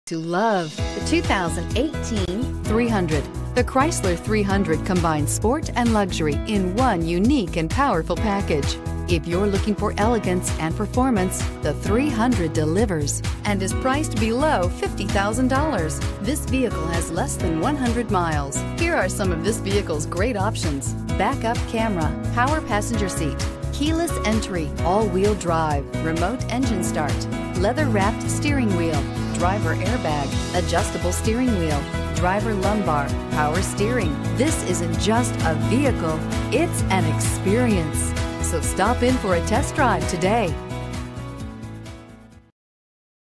2018 Chrysler 300 JH223136
2018 CHRYSLER 300 Paramus, NJ
Stock# JH223136

Delivers 27 Highway MPG and 18 City MPG! This Chrysler 300 boasts a Regular Unleaded V-6 3.6 L/220 engine powering this Automatic transmission. WHEELS: 19  X 7.5  ALUMINUM HYPERBLACK, TRANSMISSION: 8-SPEED AUTOMATIC 845RE, S MODEL APPEARANCE PACKAGE.

* This Chrysler 300 Features the Following Options *
QUICK ORDER PACKAGE 22G 300S , MAXIMUM STEEL METALLIC CLEARCOAT, MANUFACTURER'S STATEMENT OF ORIGIN, ENGINE: 3.6L V6 24V VVT, DUAL-PANE PANORAMIC SUNROOF, BLACK, VENTILATED LEATHER TRIM SPORT SEATS, 300S PREMIUM GROUP 2, 300S PREMIUM GROUP, Wireless Streaming, Window Grid Antenna.

Stop by Dodge Chrysler Jeep Ram Of Paramus located at 315 Rt 4 West, Paramus, NJ 07652 for a quick visit and a great vehicle!